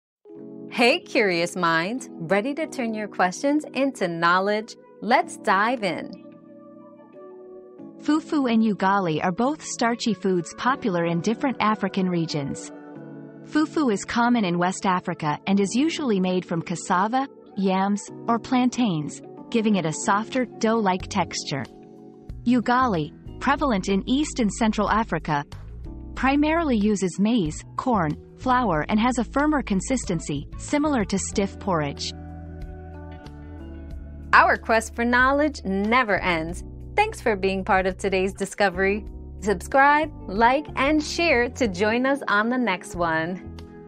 What's the difference between fufu and ugali?
Fufu vs. Ugali: Starchy Showdown 👉 Starchy Showdown 👉 Discover the key differences between fufu and ugali, two popular African starchy foods with unique textures and ingredients.

Laura S. Harris (2024, June 2.) What's the difference between fufu and ugali?
    🌐 AskAbout.video